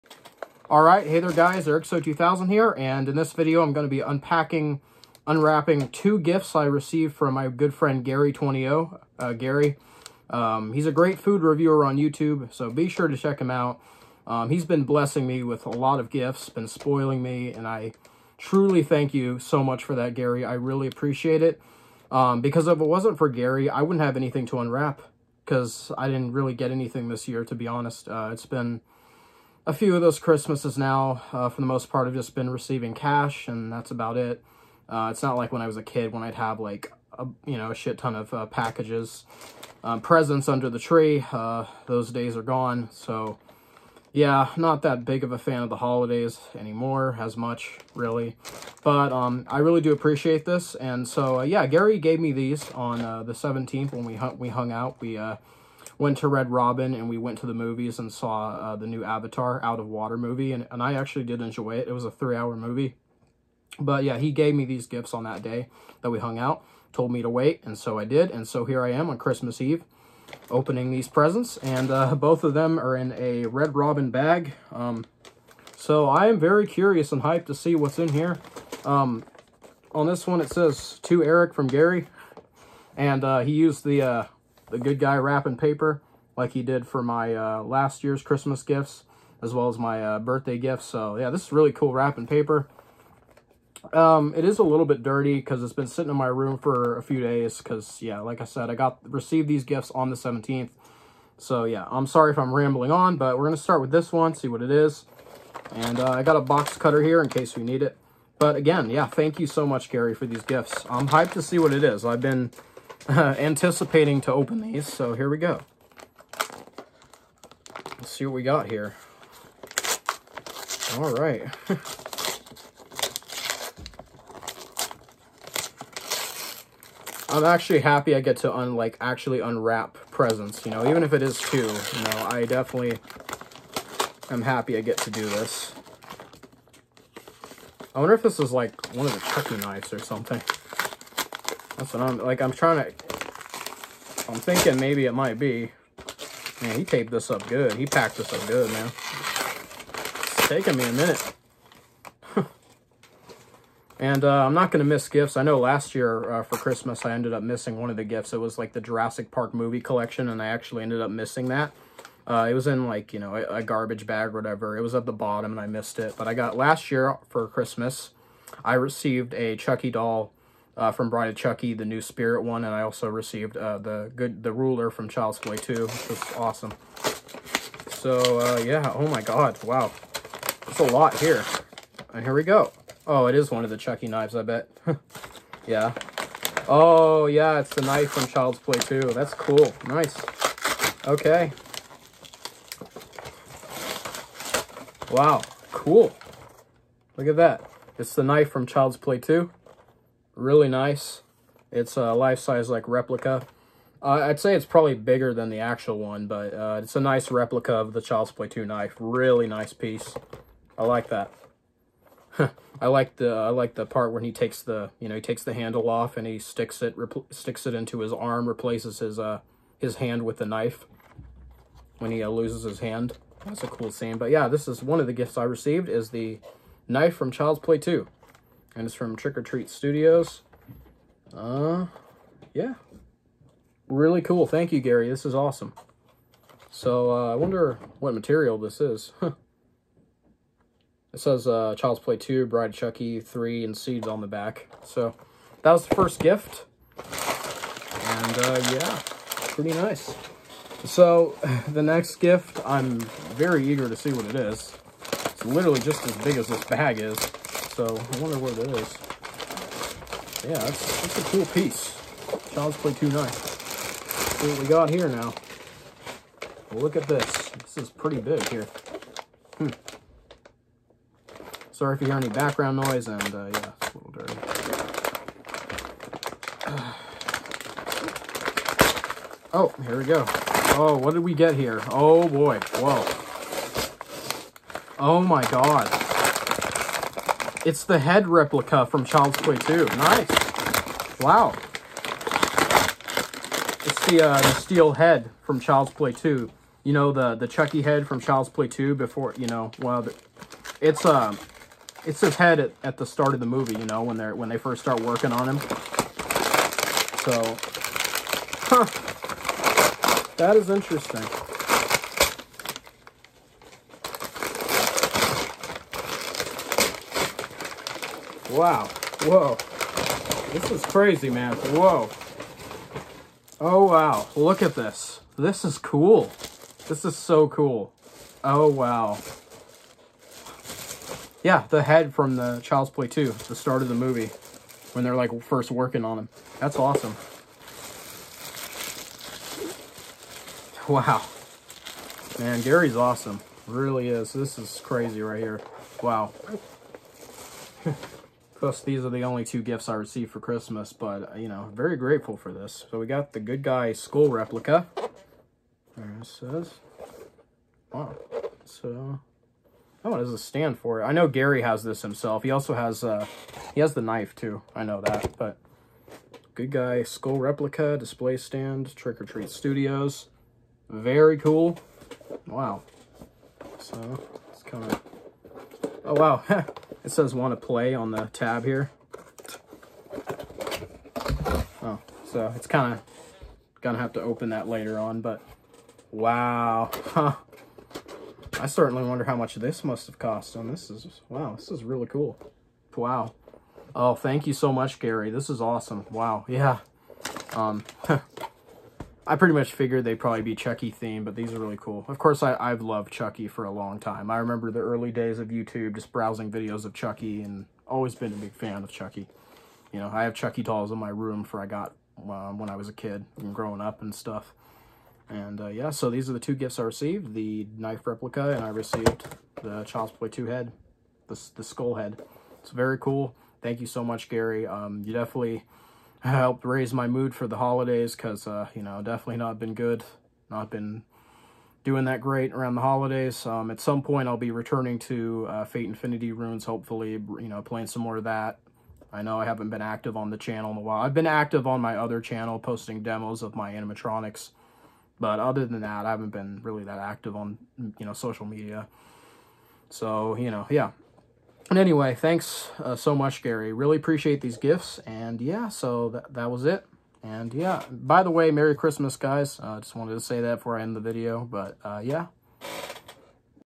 Unwrapping Christmas Gifts From Gary20o
Big thank you to Gary as always for the awesome gifts love them be sure to check him out and subscribe to him on YouTube link is down below to his channel